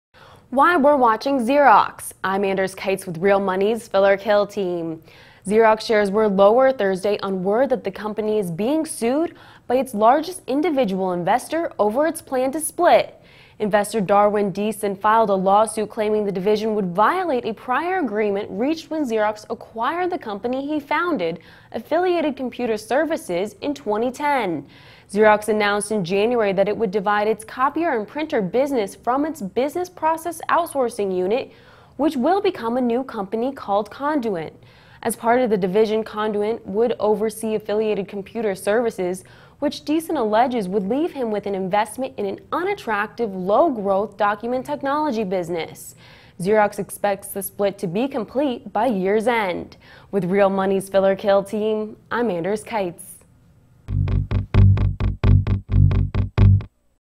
Xerox Shares Slide Over Investor Lawsuit
Xerox shares were lower Thursday on word that the company is being sued by its largest individual investor over its plan to split. Investor Darwin Deason filed a lawsuit claiming the division would violate a prior agreement reached with Xerox when it acquired the company he founded, Affiliated Computer Services, for $6.4 billion in 2010. Xerox announced in January that it would divide its copier and printer business from its business process outsourcing unit, which will become a new company called Conduent. Xerox expects the move to be complete by year's end.